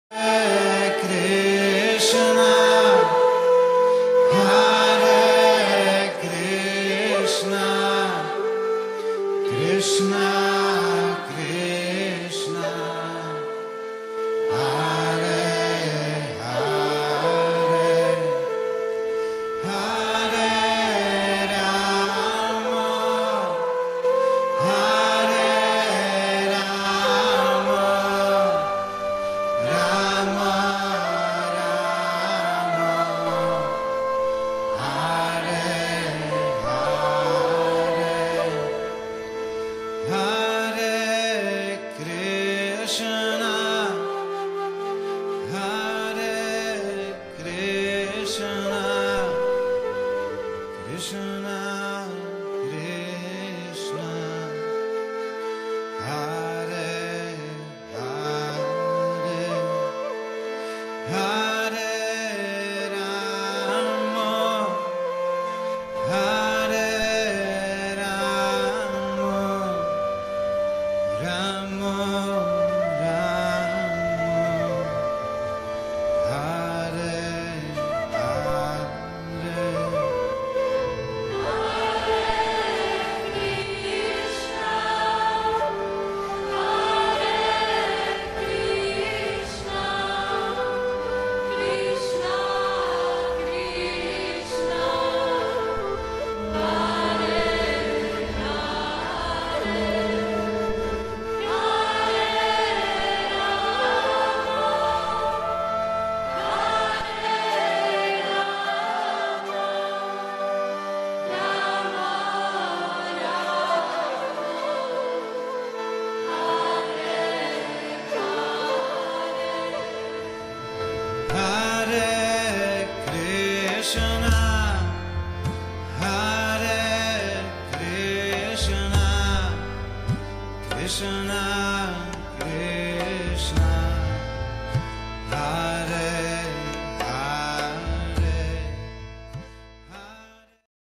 Home Deities at Krishna Chandra Prabhu's altar
Beautiful deities at the family altar of the Mafatlals.